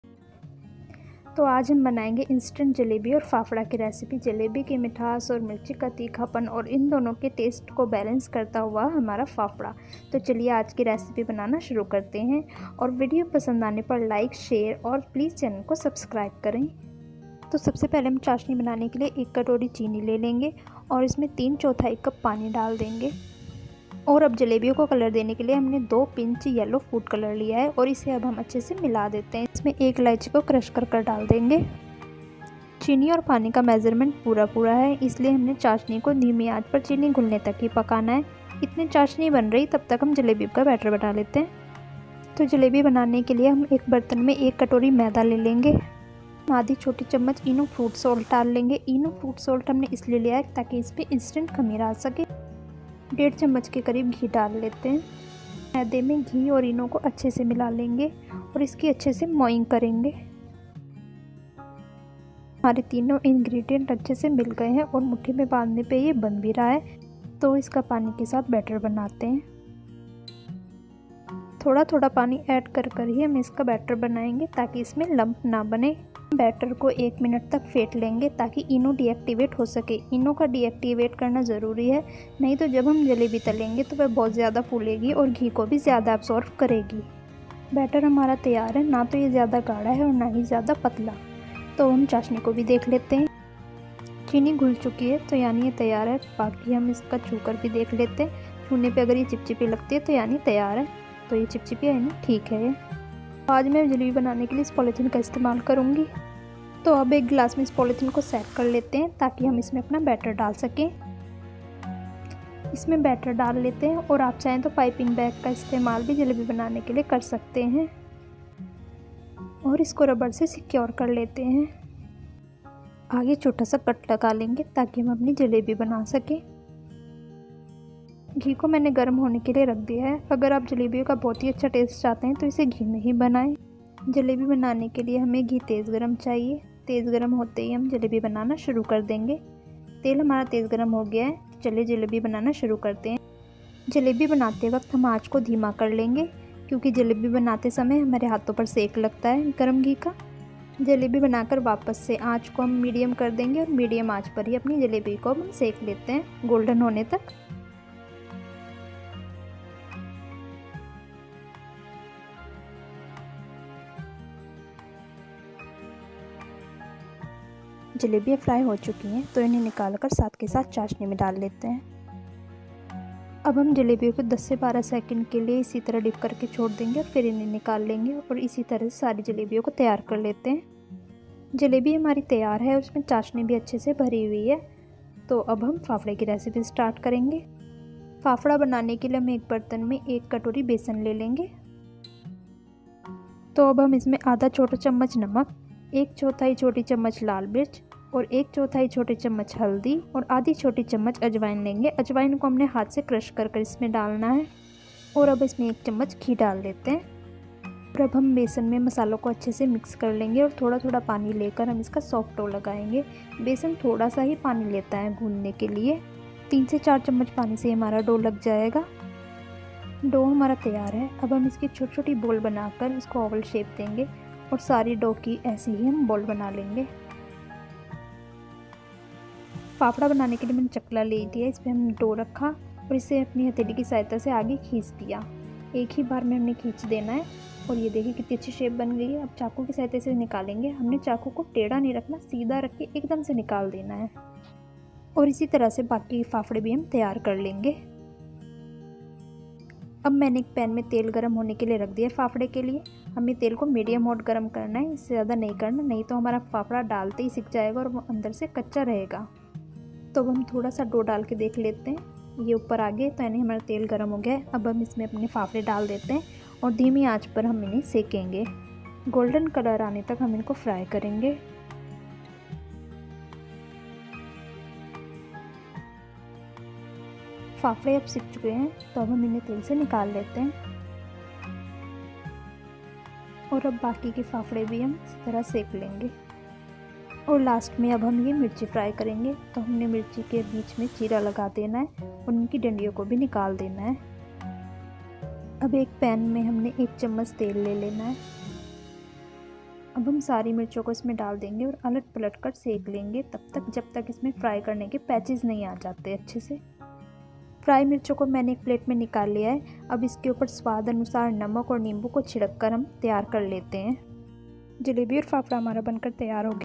Jalebi fafda recipe by M.D. Food Magic जलेबी फाफडा
This is a famous recipe of Gujrat. It has a good taste of sweet salty and spicy balance each other.